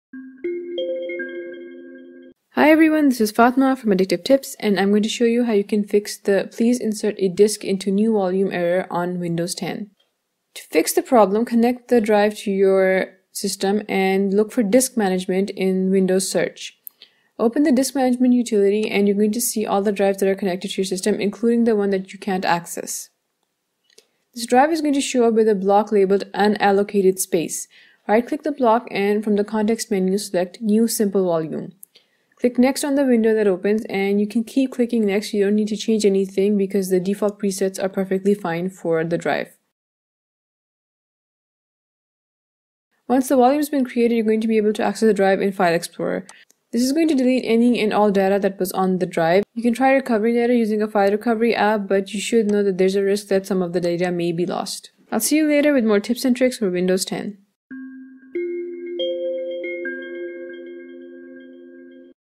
How to fix “Please insert a disk into NEW VOLUME” on Windows 10
Formatting a drive is easy enough and most end users know what a quick format, or a full format does to a disk. On Windows 10 you can format a drive from File Explorer. All you have to do is right-click the drive you want to format, and select Format from the context menu. The format options’ window lets you select the file system for the drive, and give it a name. If you’re trying to use a drive and you keep seeing the Please insert a disk into NEW VOLUME error then a format isn’t going to fix it.